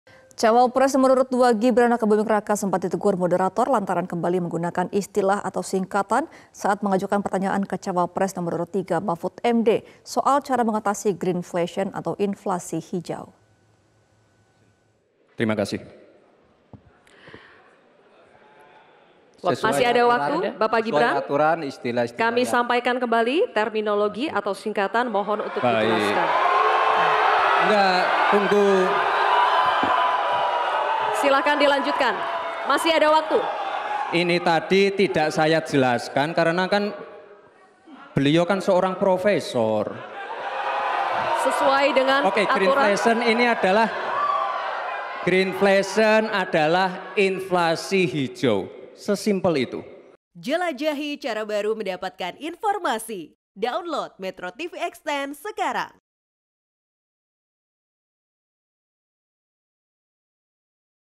Kembali Gunakan Istilah, Gibran di Tegur Moderator
MetroTV, Cawapres nomor urut dua, Gibran Rakabuming Raka, sempat ditegur moderator, lantaran kembali menggunakan istilah atau singkatan, saat mengajukan pertanyaan ke cawapres nomor urut tiga Mahfud MD, soal cara mengatasi greenflation, atau inflasi hijau.